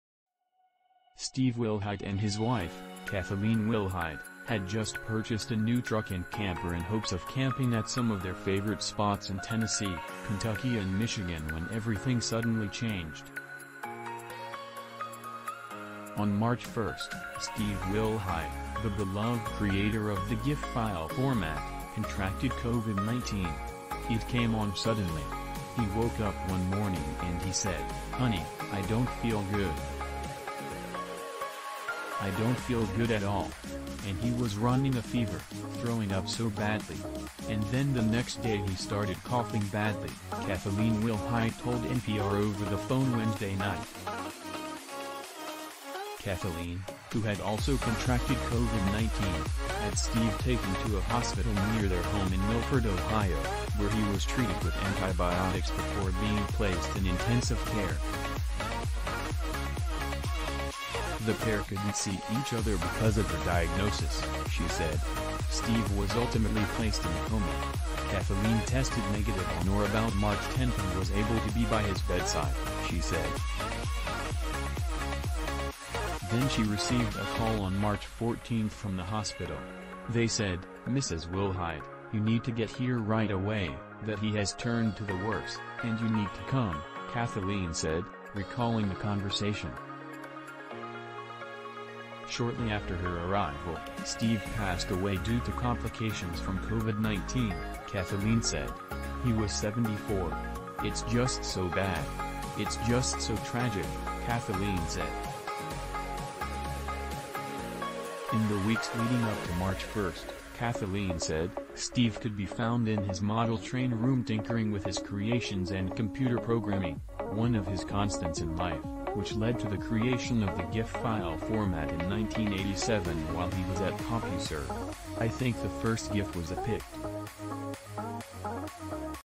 Beloved creator of the GIF dies following complications from COVID wife
Steve Wilhite and his wife, Kathaleen Wilhite, had just purchased a new truck and camper in hopes of camping at some of their favorite spots in Tennessee, Kentucky and Michigan when everything suddenly changed. On March 1, Steve Wilhite, the beloved creator of the GIF file format, contracted COVID-19."It came on suddenly. He woke up one morning and he said, 'Honey, I don't feel good. I don't feel good at all.' And he was running a fever, throwing up so badly. And then the next day he started coughing badly," Kathaleen Wilhite told NPR over the phone Wednesday night. Kathaleen, who had also contracted COVID-19, had Steve taken to a hospital near their home in Milford, Ohio, where he was treated with antibiotics before being placed in intensive care. The pair couldn't see each other because of her diagnosis, she said. Steve was ultimately placed in a coma. Kathaleen tested negative on or about March 10 and was able to be by his bedside, she said. Then she received a call on March 14 from the hospital."They said, 'Mrs. Wilhite, you need to get here right away,' that 'he has turned to the worse, and you need to come,' " Kathaleen said, recalling the conversation. Shortly after her arrival, Steve passed away due to complications from COVID-19, Kathaleen said. He was 74."It's just so bad. It's just so tragic," Kathaleen said. In the weeks leading up to March 1, Kathaleen said, Steve could be found in his model-train room tinkering with his creations and computer programming, one of his constants in life, which led to the creation of the GIF file format in 1987 while he was at CompuServe."I think the first GIF was a picture of a plane. It was a long time ago," Steve told the Daily Dot in a rare interview via Facebook in May 2012. The compressed format of the GIF allowed slow modem connections of the 1980s to transfer images more efficiently. The animation feature was added in an updated version of the GIF file format. Steve stayed at the company working on various systems until 2001, experiencing a stroke before he retired. In the 2000s, Myspace accounts were littered with buttons that glittered or scrolled by on a loop. Users of the website Tumblr employed the medium to create reactions, vignettes and memes in the 2010s. In 2013, the Webby Awards honored Wilhite with a lifetime achievement award. He played a GIF as his acceptance speech, which iterated the pronunciation as "jif," not "gif."Today, GIFs are a mainstay of communication on the internet."Without the .gif, the internet as we know it would be a different place," Jason Reed, the art director at the Daily Dot, told NPR over the Signal messaging service. "It's a tight medium that you can learn alot about storytelling within, especially tuned for the attention span of the internet."Jimmy McCain is the co-founder of the artist collective known as Mr. GIF, known best for, well, creating GIFs."I want to offer my sincerest condolences to Stephen Wilhite's family. It is incredibly saddening to hear.